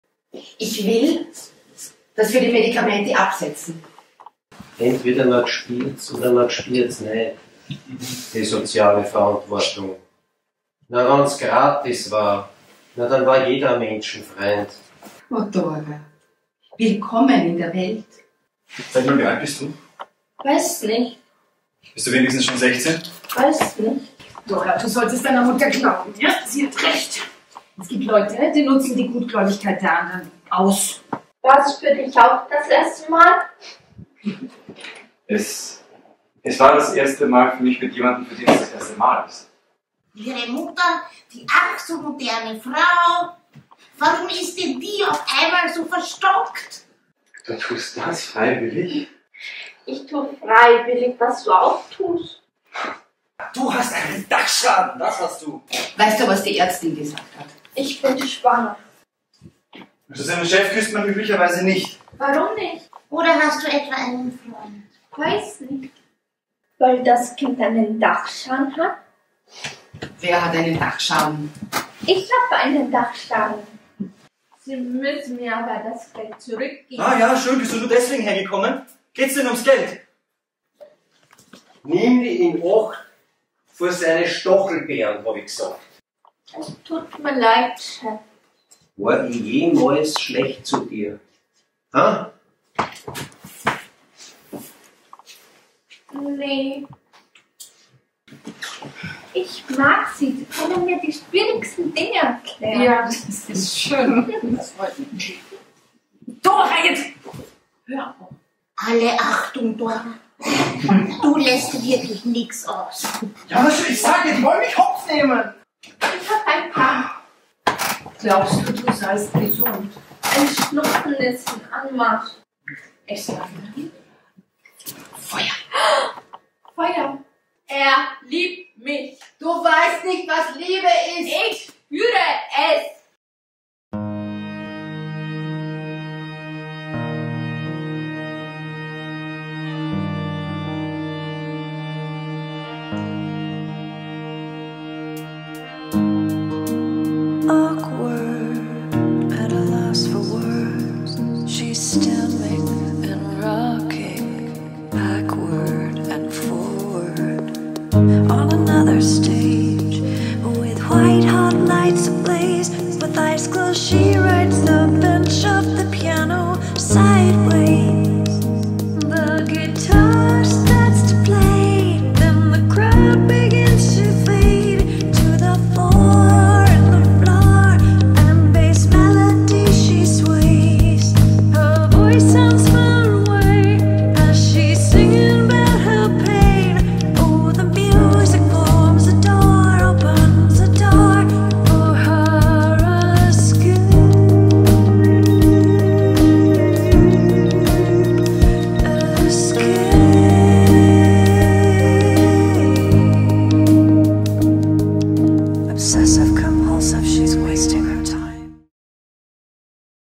Theater Delphin: Dora oder Die sexuellen Neurosen unserer Eltern
Dora
oder Die sexuellen Neurosen unserer Eltern

Schauspiel: Heide-Maria Hager, Silvana Filipovic,
Gabriele Weber, Georg Wagner, Ivana Veznikova, Filipp Peraus

Regie: Nico Wind
Produktionsleitung: Georg Wagner
Künstlerische Leitung: Gabriele Weber
Regieassistenz: Jona Haidenthaler
Kostüme: Sigrid Dreger
Bühnenbild: Georg Wagner
Foto: Georg Wagner
Kamera: Pithawat Limwattanaphanchai
Graphik / Schnitt: Eva Seidl

Verlag Wallstein | Uraufführung Theater Basel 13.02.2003

Eine Produktion von Theater Delphin
23.–25. November 2023
30. November – 2. Dezember 2023